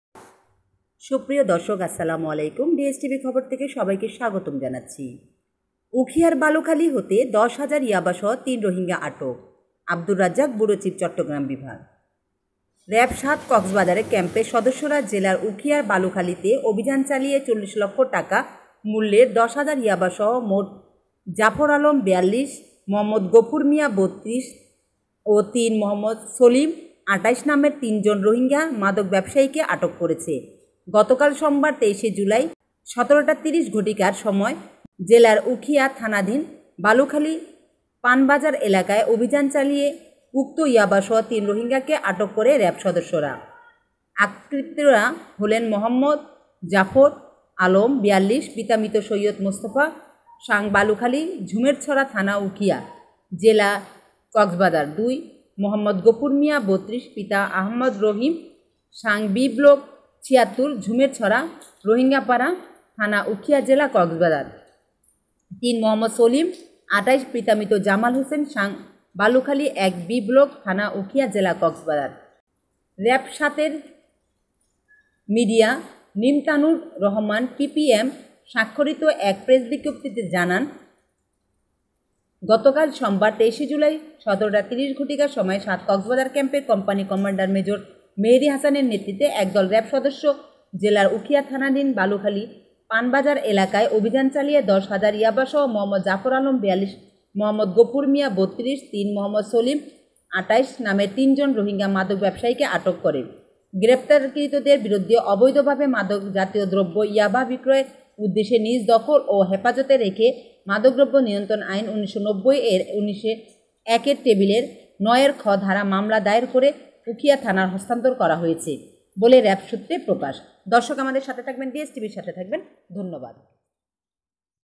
উখিয়ার বালূখালী হতে ১০ হাজার ইয়াবাসহ ৩ রোহিঙ্গা আটক
আবদুর রাজ্জাক,ব্যুরো চীফ,চট্টগ্রাম বিভাগ
র‌্যাব-৭’কক্সবাজার ক্যাম্পের সদস্যরা  জেলার উখিয়ার বালুখালীতে অভিযান চালিয়ে ৪০ লক্ষ টাকা মূল্যের ১০ হাজার ইয়াবাসহ মোঃ জাফর আলম (৪২),মোঃ গফুর মিয়া (৩২) ও (৩) মোঃ ছলিম (২৮) নামের ৩ জন রোহিঙ্গা মাদক ব্যবসায়ীকে আটক করেছে। গতকাল সোমবার (২৩ জুলাই) ১৭.৩০ ঘটিকার সময় জেলার উখিয়া থানাধীন বালুখালী পানবাজার এলাকায় অভিযান চালিয়ে উক্তইয়াবাসহ ৩ রোহিঙ্গাকে আটক করে র‌্যাব সদস্যরা।